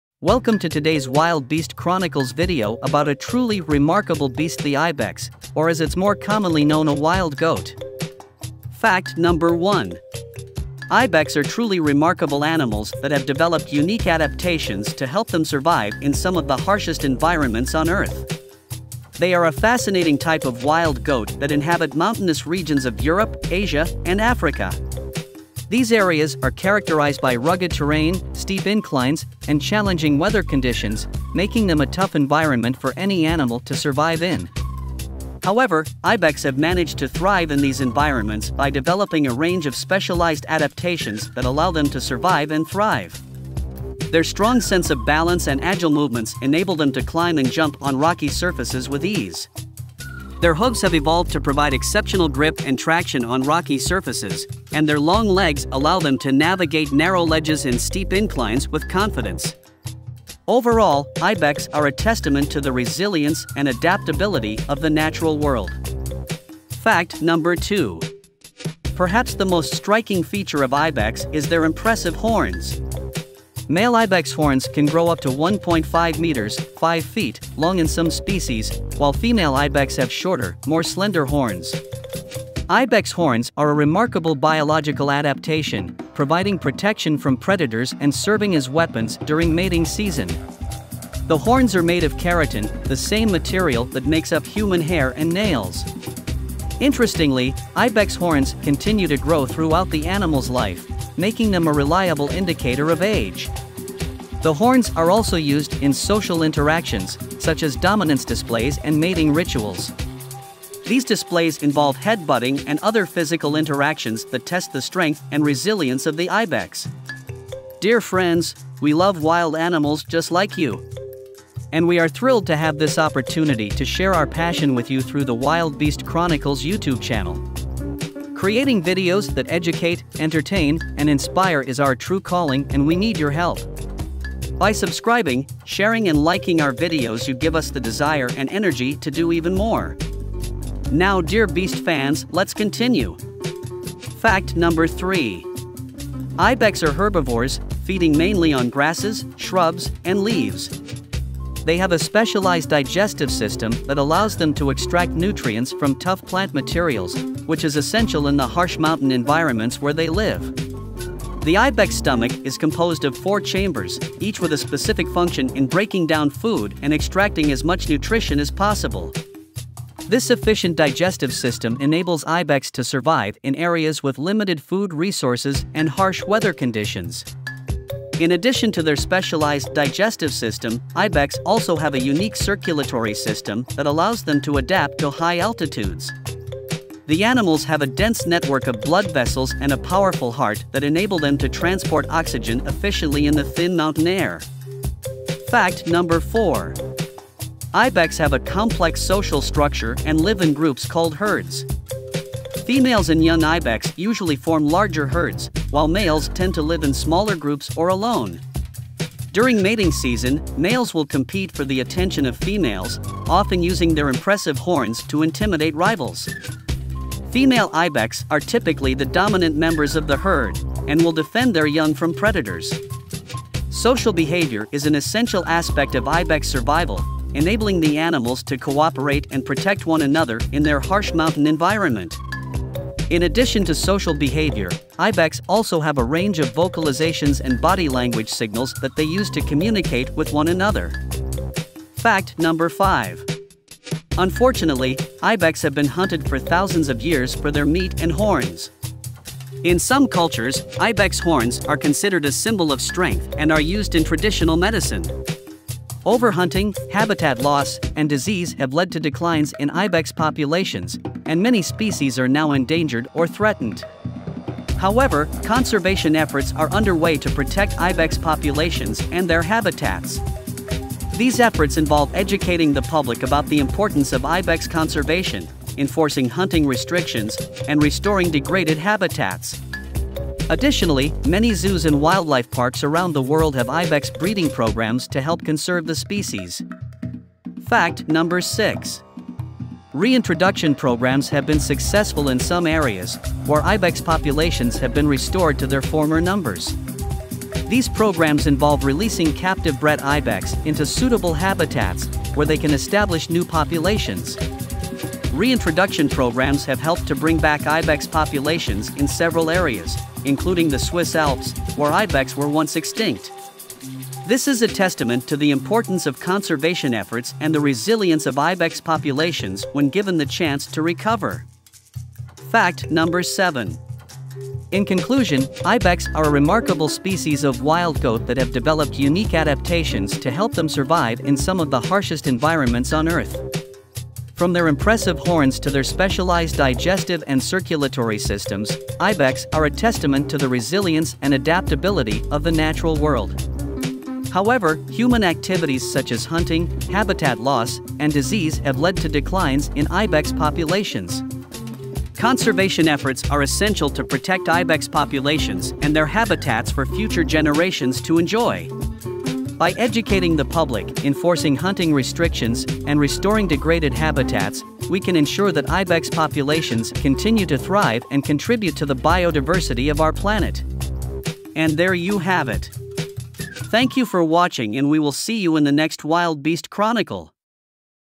Ibex : 7 Common and STRANGE facts
Ibex are truly remarkable animals that have developed unique adaptations to help them survive in some of the harshest environments on Earth.

If you enjoy this video please consider some of our other videos here on youtube!